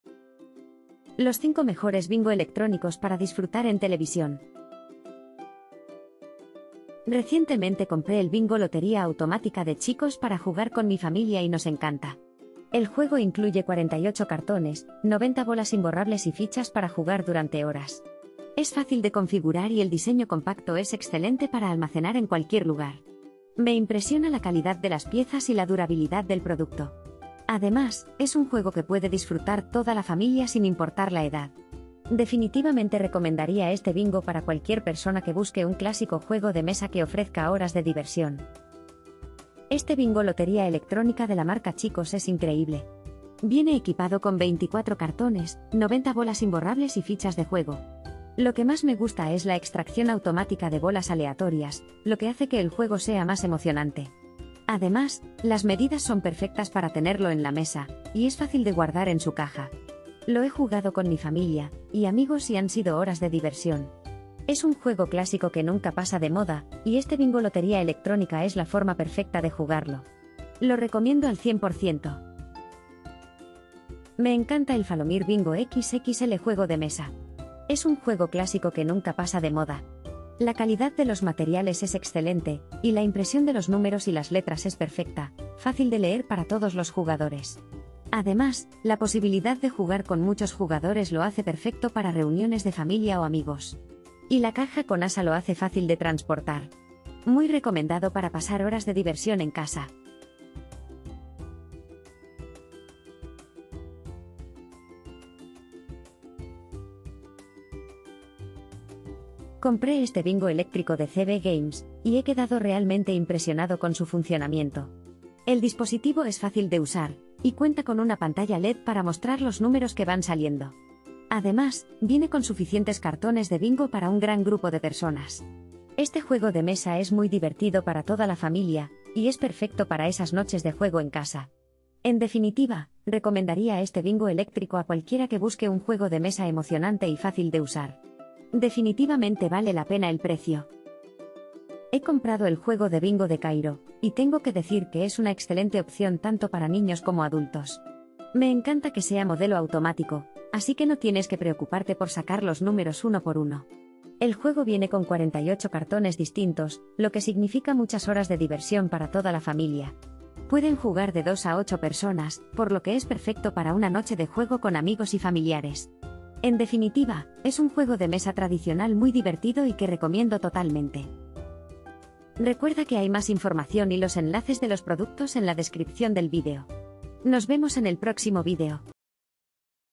【BINGO ELECTRONICO TV】Los 5 mejores bingo electrónicos para disfrutar en televisión
✋  Aquí están los 5 mejores bingo electrónicos para disfrutar en televisión disponibles en el mercado.

🔽🔽🔽 ENLACES DE COMPRA BINGO ELECTRONICO TV 🔽🔽🔽

1) Chicos - Bingo Lotería automática | Incluye 48 cartones, 90 Bolas imborrables y fichas de Juego |Un Juego de Siempre para Toda la Familia | Medidas: 235 x 310 x 17 mm (20805)

2) Chicos - Bingo Lotería Electrónica con 24 Cartones, 90 Bolas imborrables y fichas de Juego | Extracción automática de Bolas aleatorias | Medidas: 22.5 x 37 x 10.5 cm (22302)

3) Falomir- Bingo XXL Juego De Mesa, Multicolor, Talla Única (23030)

4) CB GAMES - Bingo eléctrico juegos de mesa CB Games (25680)

5) Cayro - Bingo - + 6 Años - Modelo Automático - Juego Divertido de Mesa - para Niños y Adultos - Incluye 48 Cartones Diferentes - Ideal 2 a 8 Jugadores

00:00 - Intro
00:05 - 1) Chicos - Bingo Lotería automática | Incluye 48...
00:38 - 2) Chicos - Bingo Lotería Electrónica con 24 Cartones,...
01:13 - 3) Falomir- Bingo XXL Juego De Mesa, Multicolor, Talla...
01:48 - 4) CB GAMES - Bingo eléctrico juegos de mesa...
02:20 - 5) Cayro - Bingo - + 6 Años - Modelo Automático - Juego...
02:51 - Outro